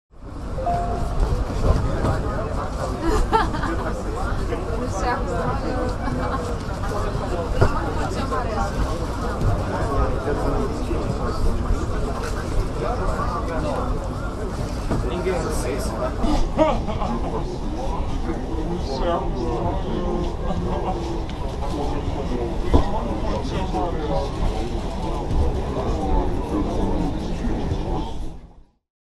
FIFA World Cup 2014 Felipao tackles Dani Alves Camp in Teresopolis
During the Brazil training session at their base camp in Teresopolis